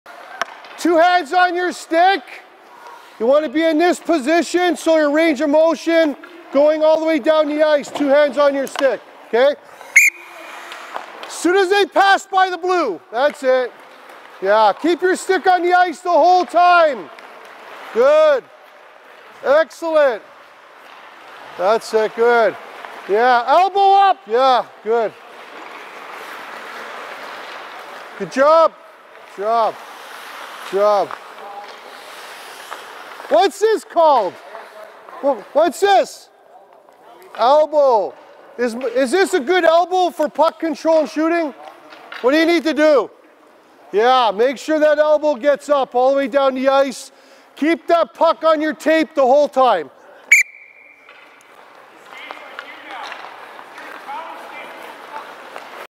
Video Tip - Puck Release Position Elbow Up.
𝗩𝗶𝗱𝗲𝗼 𝗧𝗶𝗽 - When skating in a straight line while keeping and carrying the puck in a prepared/loaded shot release position, having an elevated grip arm elbow and maintaining a low center of gravity (posture), can help to expand the space (gap) required for the grip (top) hand range of motion needed to control, pass or shoot the puck.